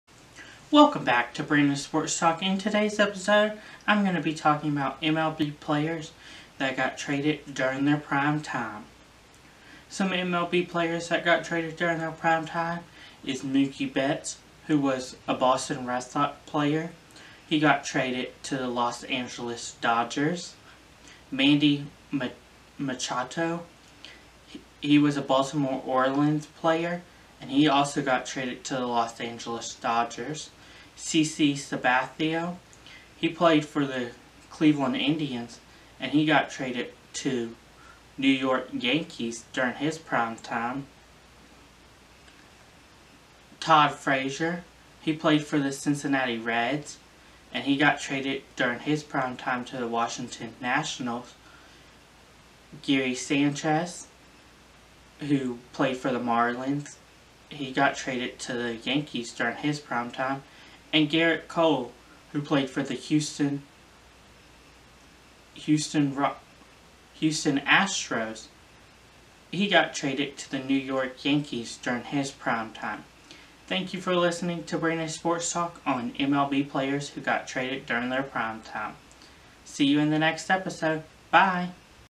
MLB Players got traded during thier prime time
In this viedo I talk about  MLB Baseball players that got trade to different teams during their prime time season's. Gerrit Cole Houston Astros to New York Yankees,  Mookie Bettes the Boston Redsox player got traded in his prime to the Los Angeles Dodgers, Manny Machado Baltimore Orioles to Los Angeles Dodgers, CC Sabitha Cleveland Indians to New York Yankees, Todd Frazier Cincinnati Reds to Washington Nationals, Giancarlo Stanton Marlins to Yankees.